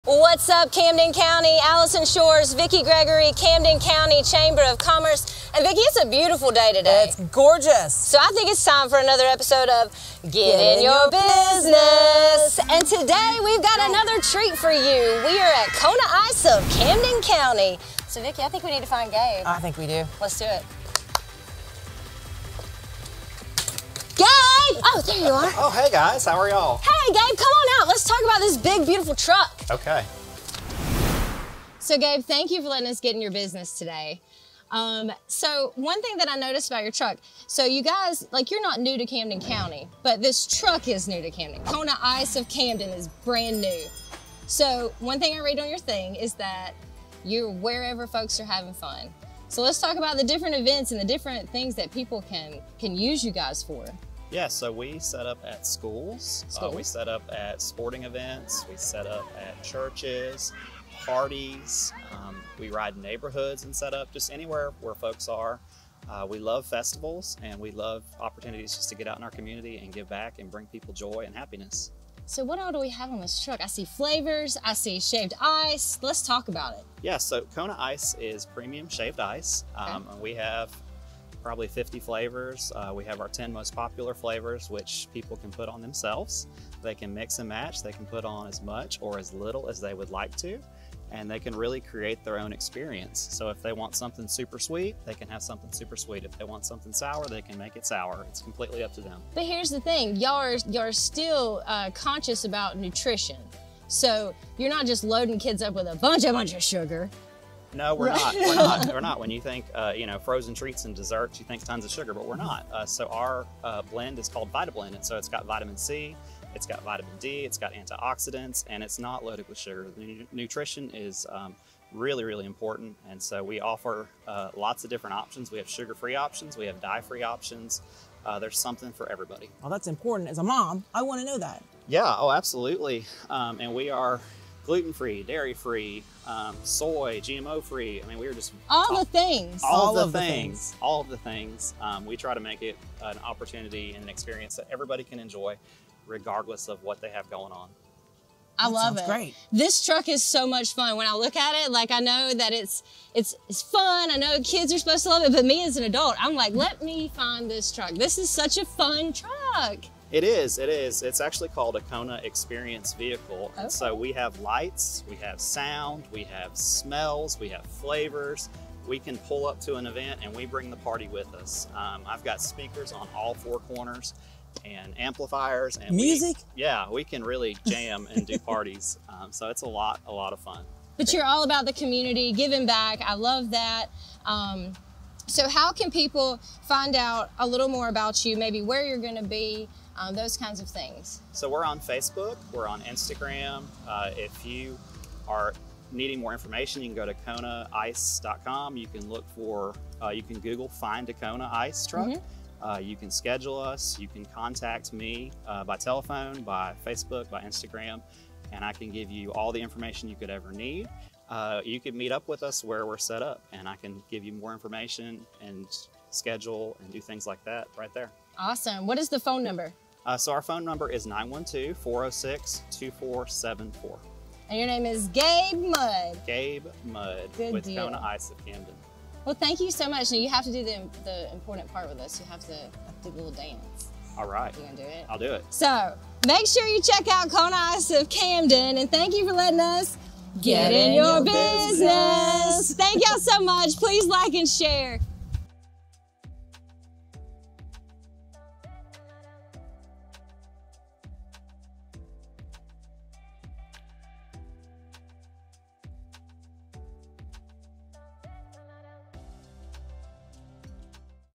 Kona Ice Of Camden | Get 'N' Your Business
On this week's episode of Get 'N' Your Business! We interview Gabe from Kona Premium Shaved Ice! With their one-of-a-kind, gourmet shaved ice experience! Kona ice brings the party to you and yours! Anywhere there is fun Kona Ice is right there with you!